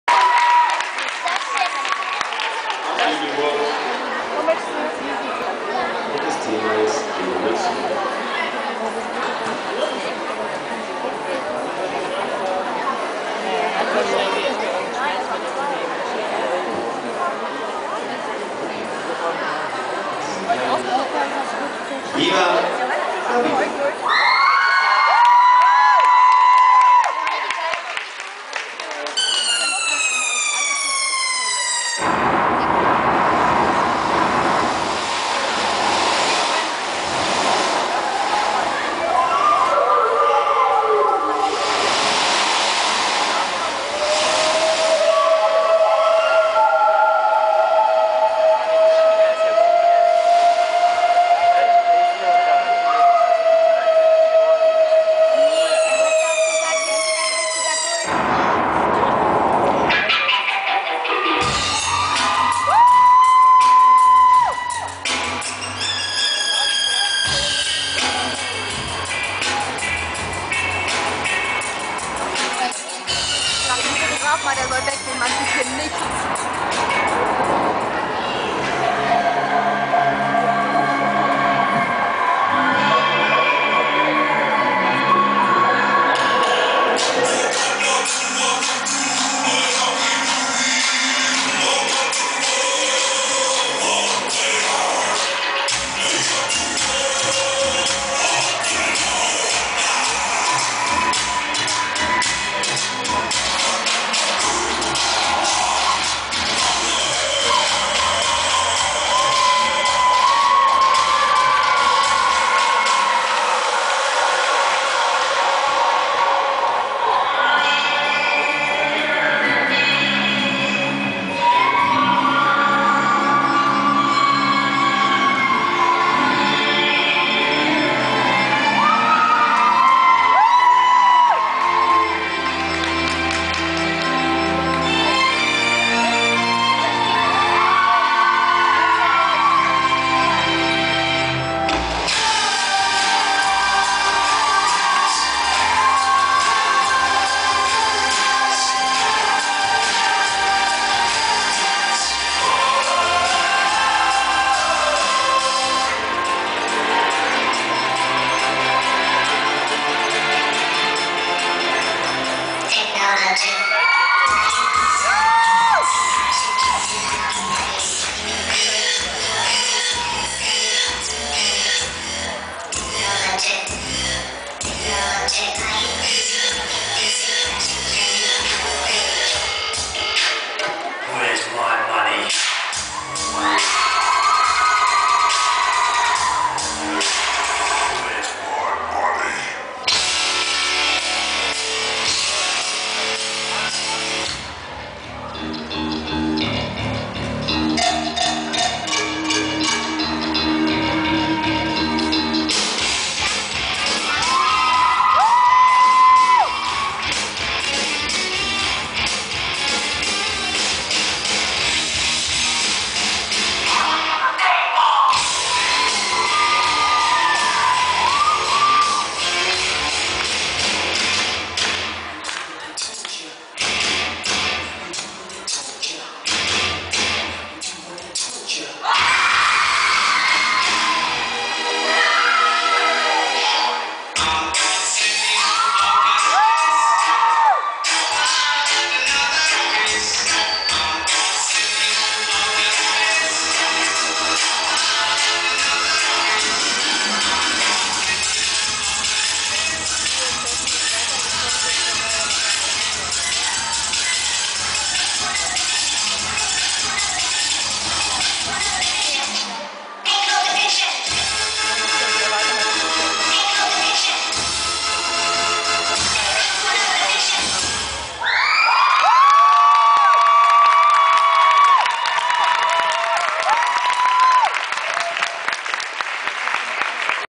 Viva-la-Vida. "Evolution" bei der Dance2U 2014 in Wolfenbüttel.
1. Platz (fortgeschrittene Adults) für Viva-la-Vida vom VfL Lüneburg!